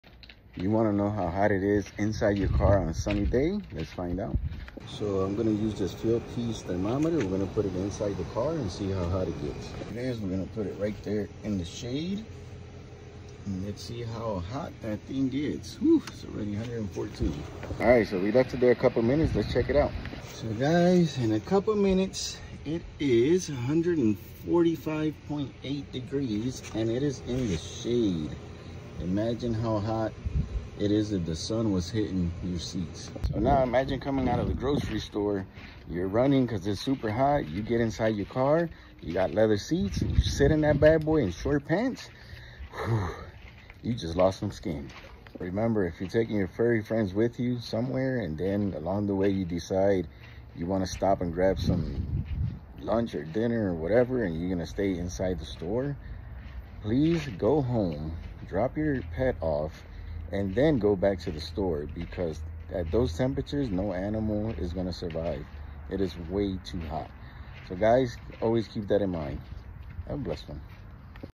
Testing the Extreme Temperatures Inside a Parked Car
Ever wondered how hot it can get inside your vehicle on a scorching day? We're talking about the kind of heat that'll make you sweat just thinking about it! In this video, we're going to put the heat index to the ultimate challenge, measuring the car temperature to find out just how hot it can get. From the sun-baked seats to the sweltering dashboard, we'll take you on a journey that'll make you think twice about leaving your car parked in direct sunlight. So, buckle up and get ready to learn some valuable heat awareness tips to keep you safe on the road!